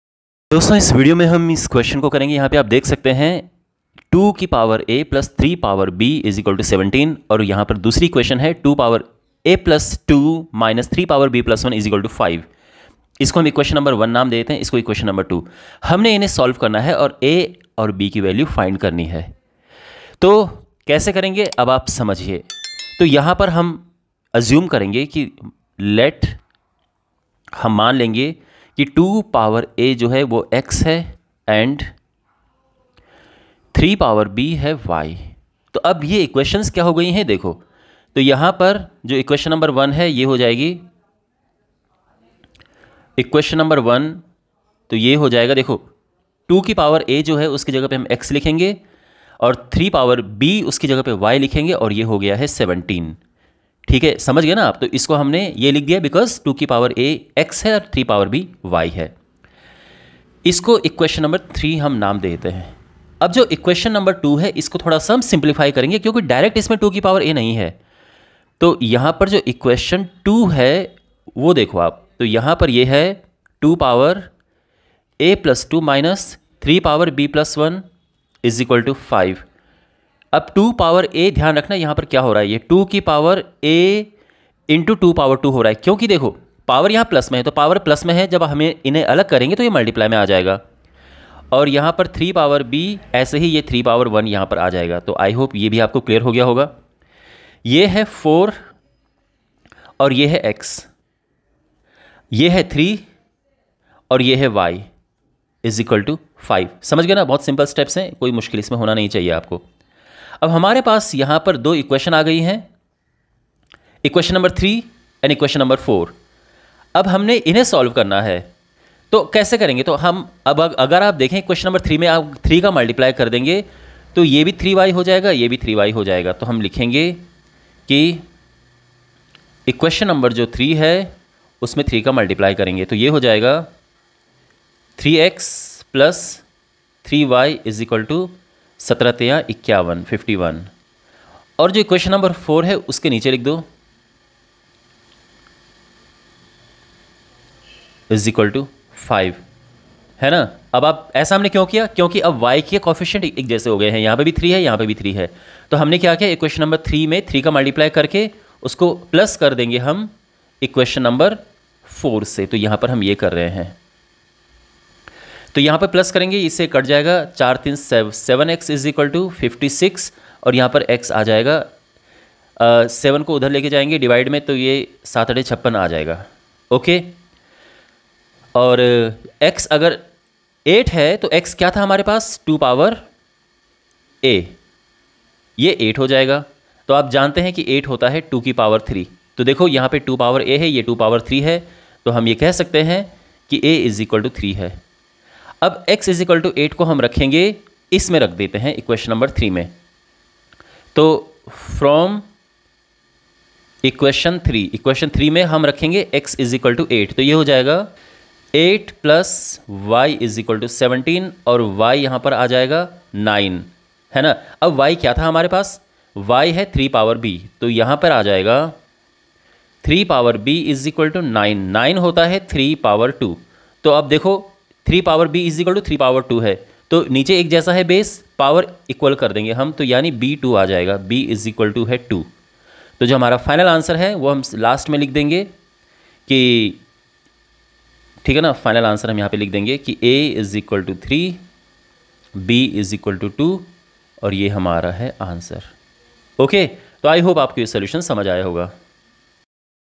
Solve for a and b, if 2^a + 3^b = 17 and 2^a+2 - 3^b + 1 = 5 | Solving pair of equations | JP Sir
Solve for a and b
if 2^a + 3^b = 17 and 2^a+2 - 3^b + 1 = 5
Solving pair of equations
JP Sir

Maths Topics by JP Sir